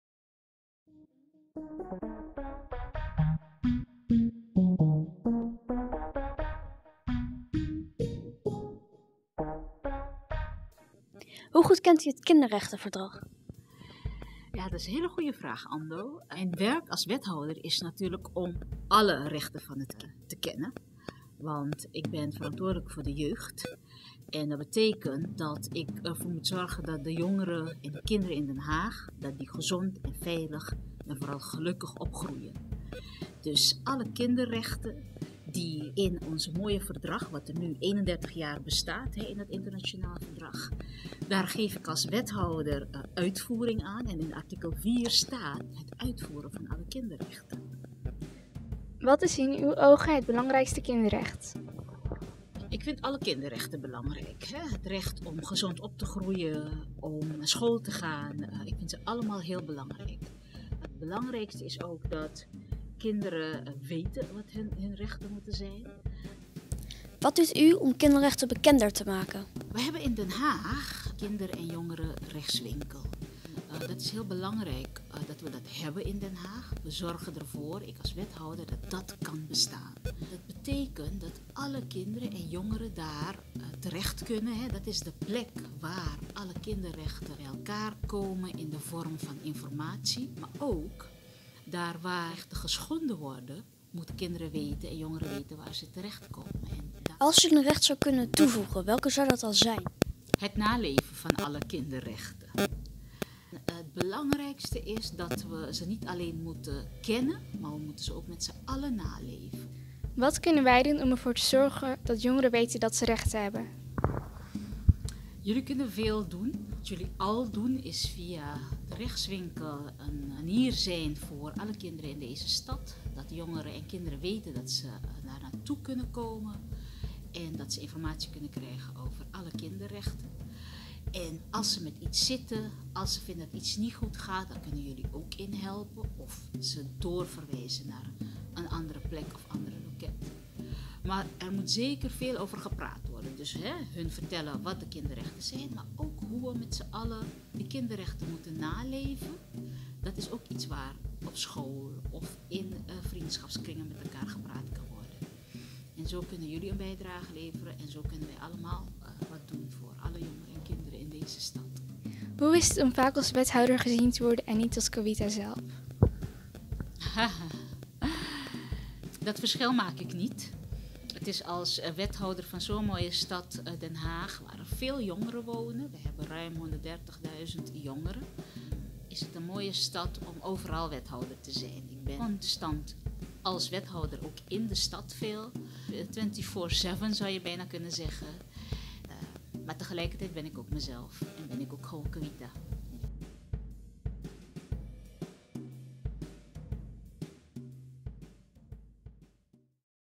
Wat weet de Haagse wethouder Kavita Parbhudayal van kinderrechten?
In deze video onderzoekt de Haagse kinderredactie wat wethouder Kavita Parbhudayal weet van kinderrechten.